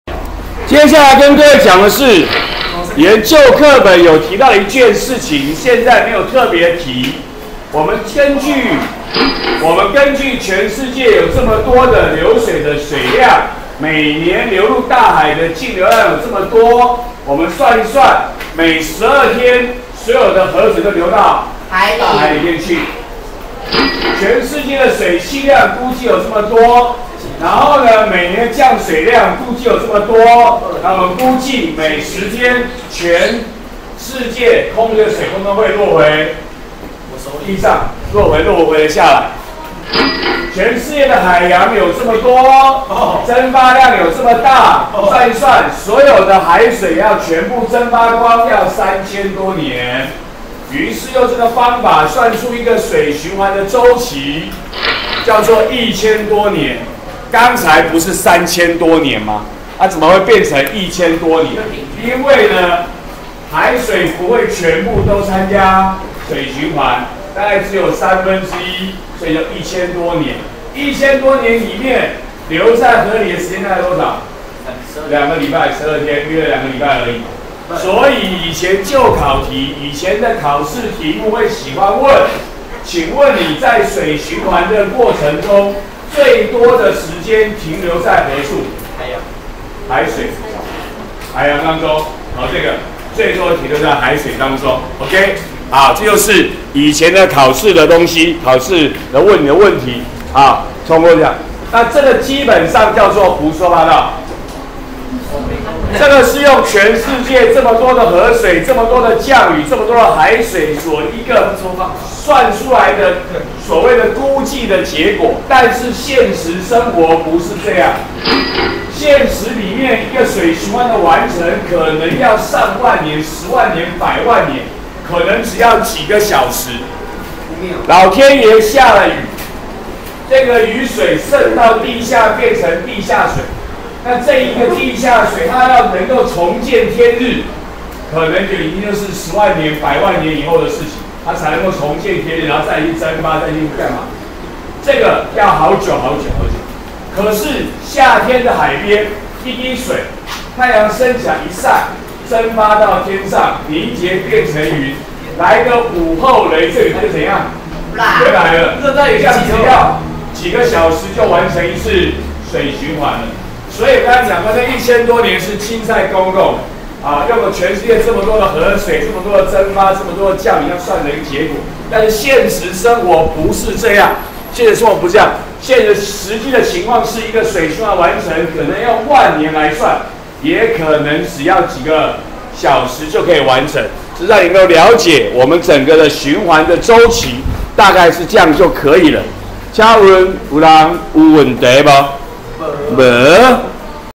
104學年地科＿水圈05：循環週期、停留時間
介紹水的循環週期，及在各式水體停留時間。